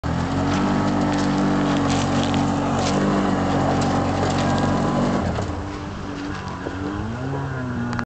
yamaha rhino stuck
havasu. tried to go down to the watter. almost didnt make it out.